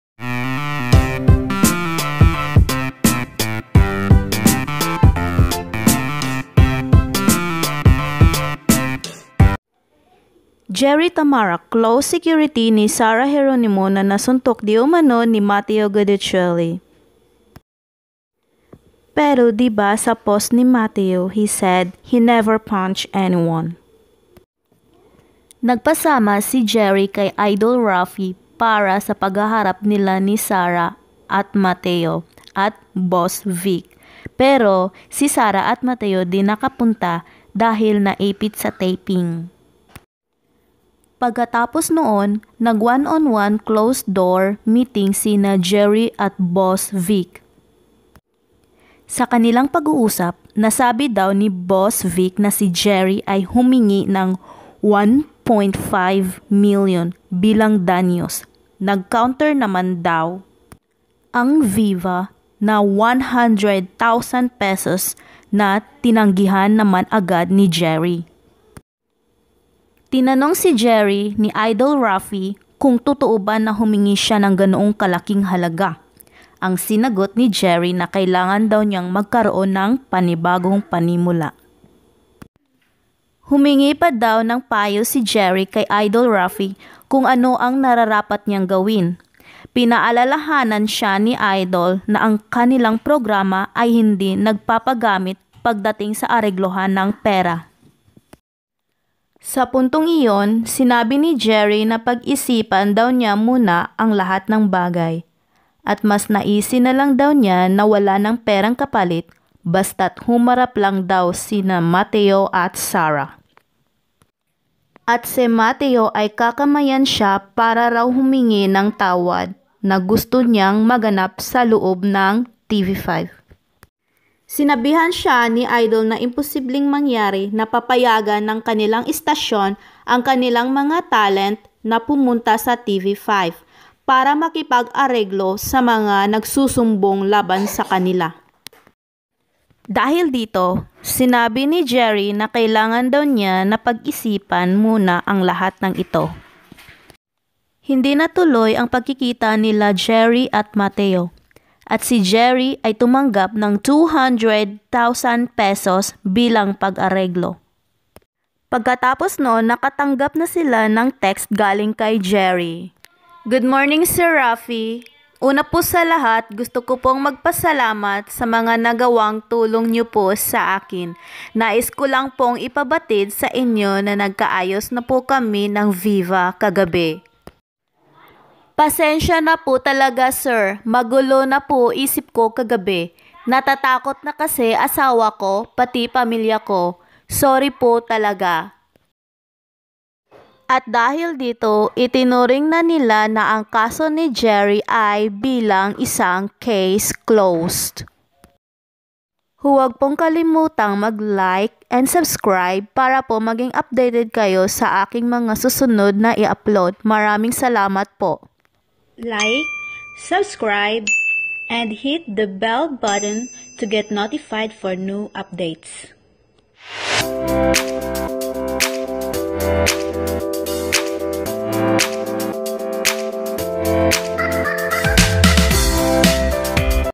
Sumbong ng BodyGuard ni Sarah G. CASE CLOSED na
Mabuti naman at na Case Closed na ang sumbong ng Body Guard ni Sarah G. laban kay Matteo G.
No more stressed na para sa bagong Kasal. At sana matanggap na ni Mommy Divine na ang kanyang anak ay may Asawa na at mayroon na siyang Son In Law.